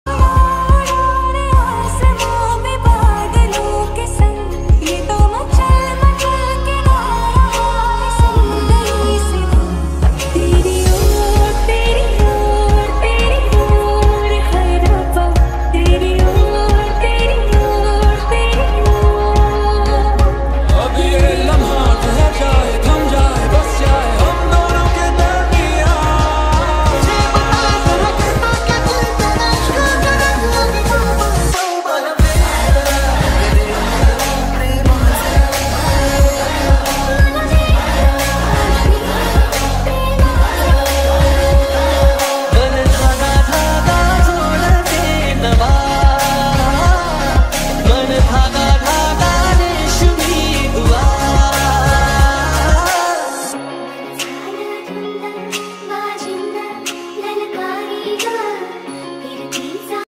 simple glam makeup | creating eye look using mars blush pallette#makeup
products used - concealer hilary rhoda
powder - Maybelline fit me compact
Mars blusher pallette
glam 21 eyeliner
colossal kajal
Mars highlighter